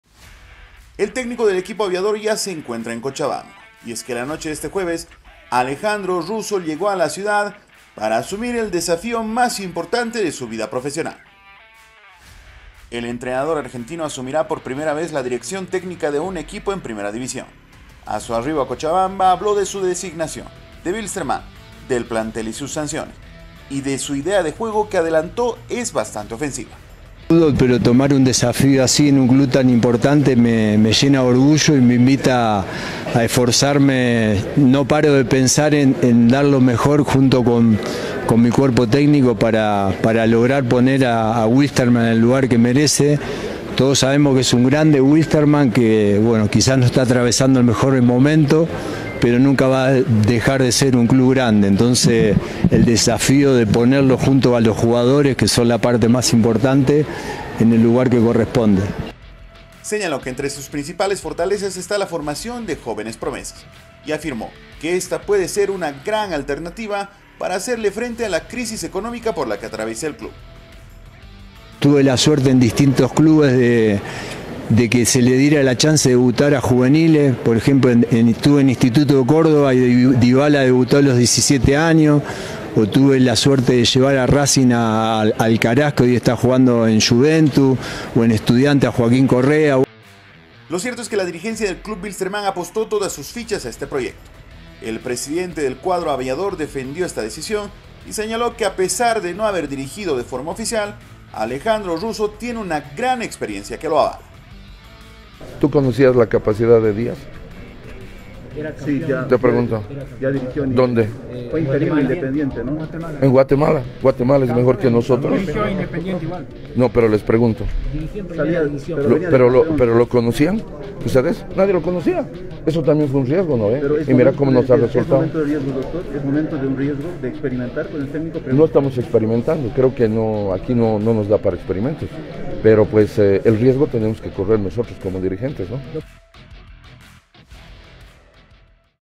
RUSSO LLEGÓ A COCHABAMBA PARA ASUMIR EL DESAFÍO MÁS IMPORTANTE DE SU CARRERA
El argentino, elegido por la dirigencia para ser el técnico del equipo aviador, señaló que espera aplicar todos sus conocimientos para devolver a Wilstermann al sitio en el que se merece, habló de la hinchada y del plantel y de su idea de juego ofensiva, en medio de las críticas la dirigencia defiende su designación.